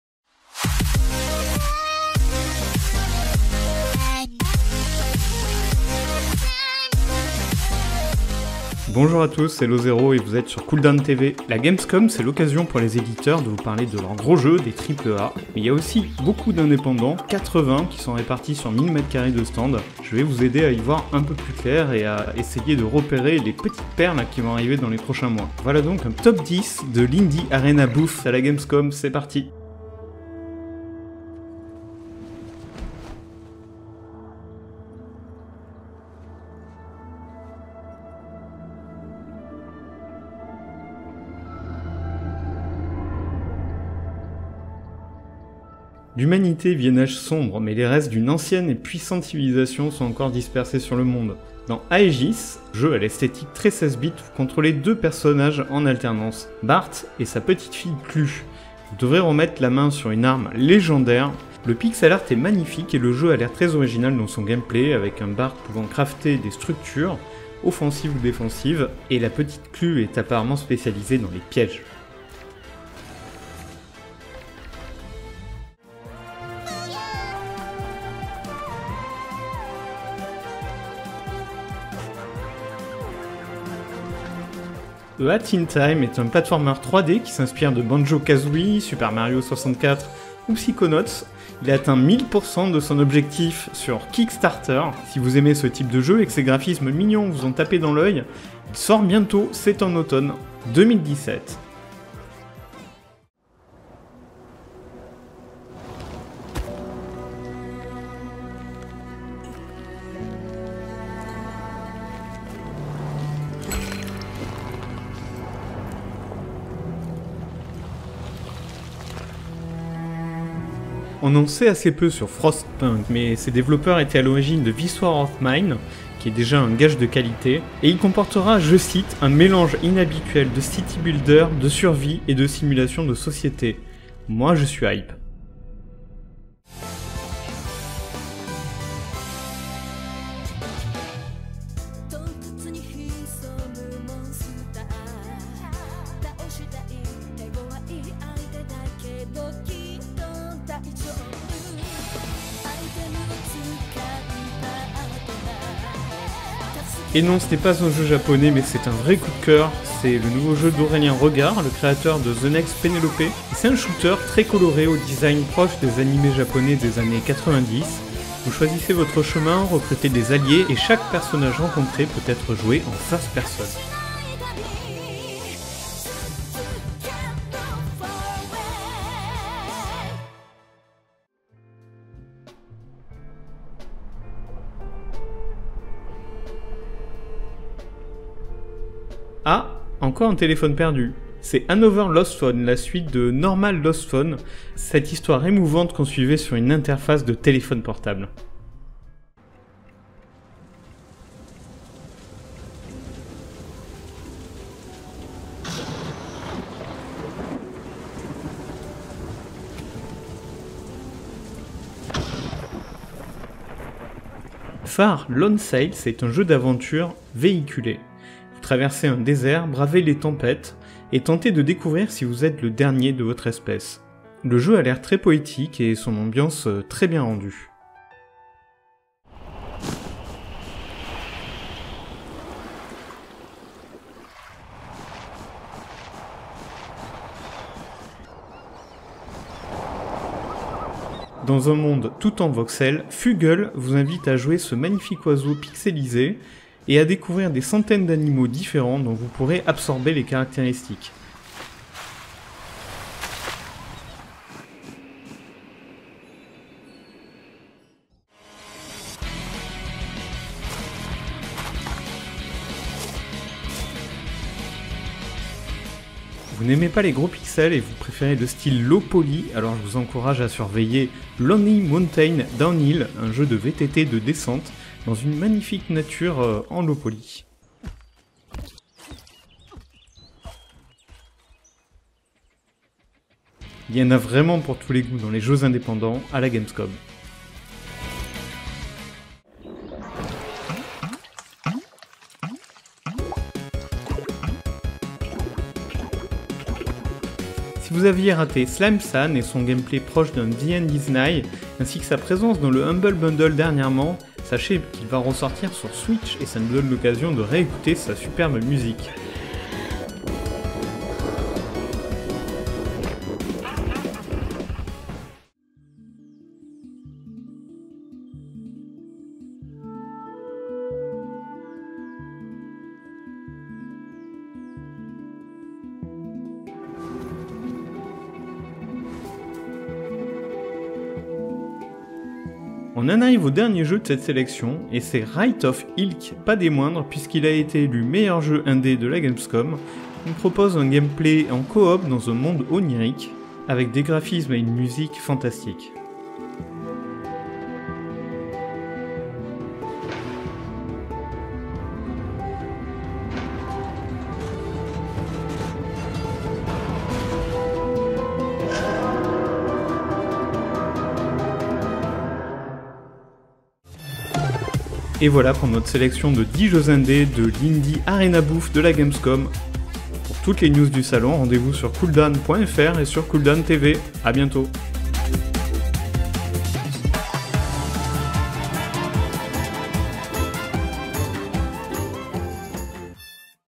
[CDTV] Notre sélection de 10 jeux indépendants de la Gamescom 2017 !
Si vous voulez vous y retrouver dans les 80 jeux présentés cette année sur le stand "Indie arena" de la Gamescom, nous avons sélectionné pour vous 10 jeux à suivre absolument !